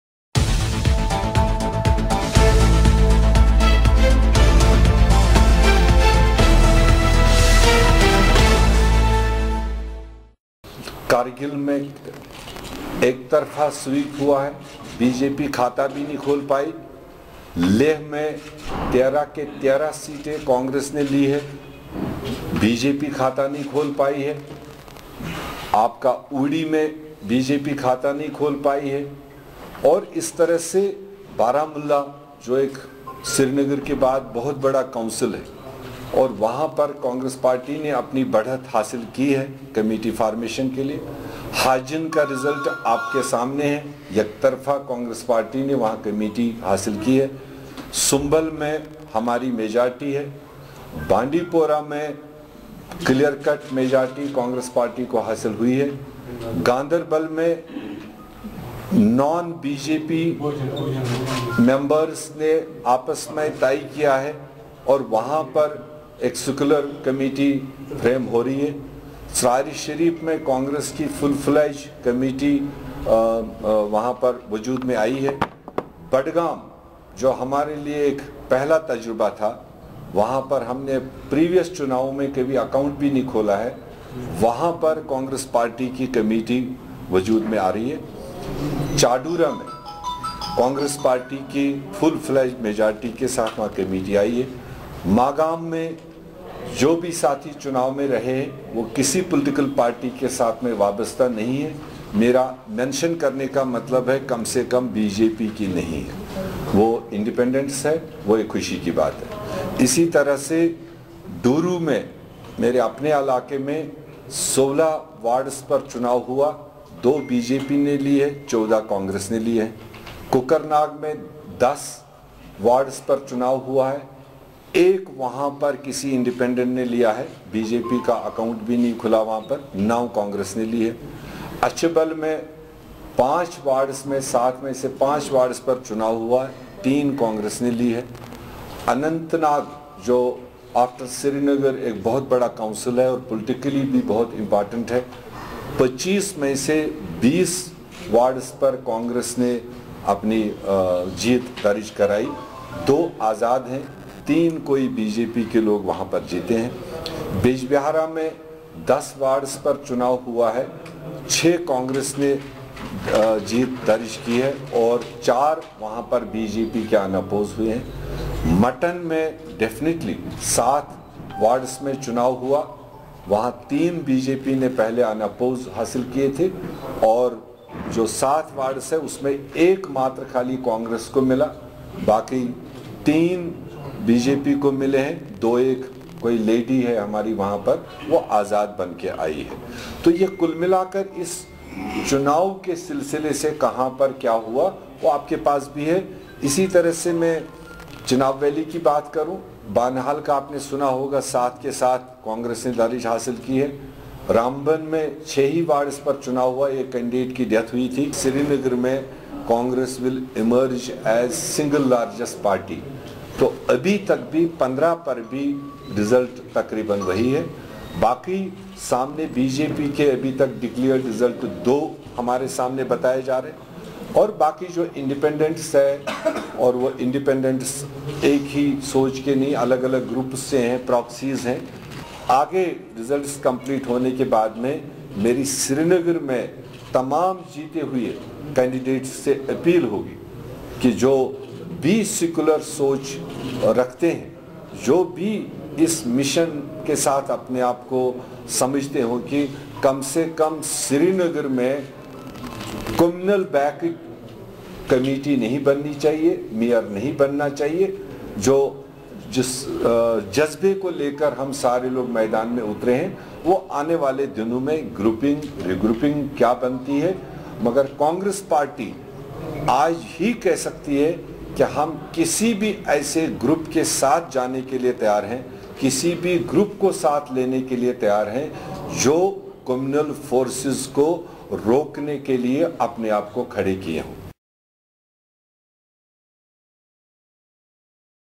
Video: People reject BJP-RSS in recently held ULB polls: G A Mir
Says “BJP was hell bent upon to grab power in Valley”